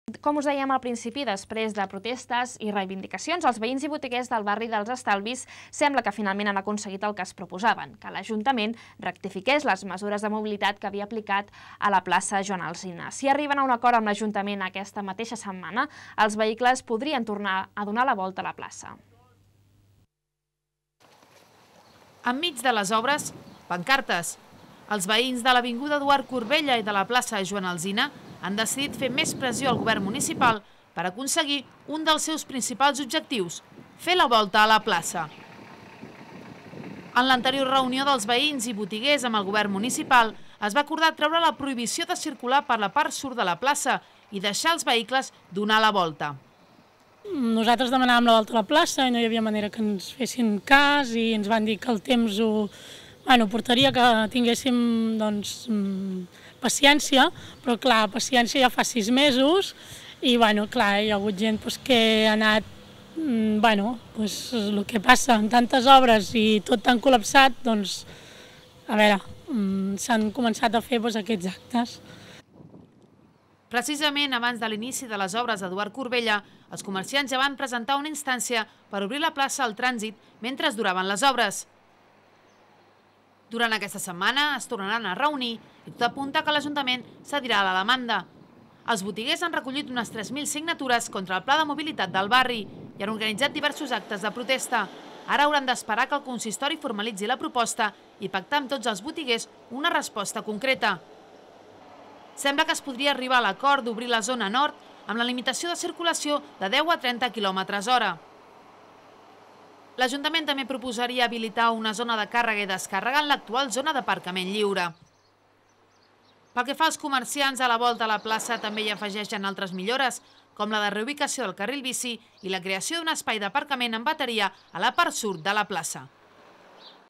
01 PANCARTES A L'ESTALVI
Després de protestes i reivindicacions els veïns i botiguers del Barri dels Estalvis sembla que finalment han aconseguit el que es proposaven: que l'ajuntament rectifiqués les mesures de mobilitat que havia aplicat a la plaça Joan Alsina.
Si arriben a un acord amb l'equip de govern aquesta setmana, els vehicles podrien fer la volta a la plaça.